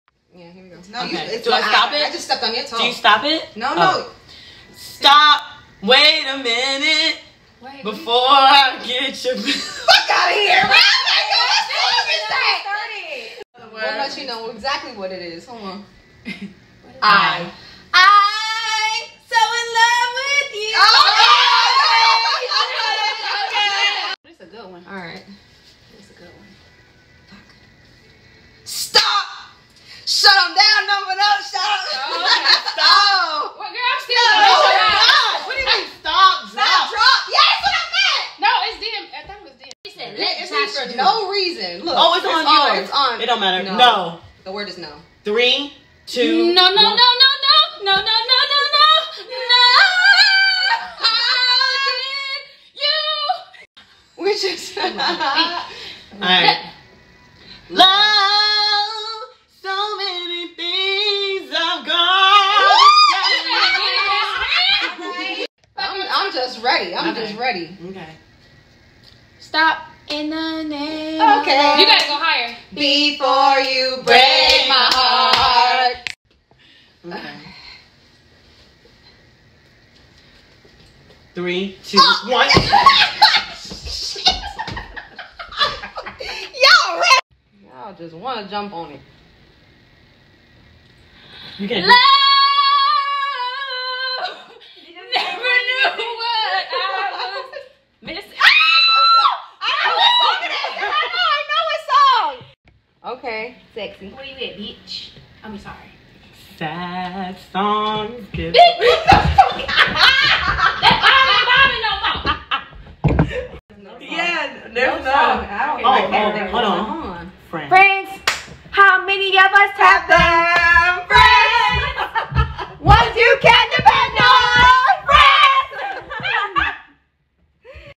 Sing A Song With The Word Challenge
Sing A Song With The Word Challenge X Pillow Fight with the girls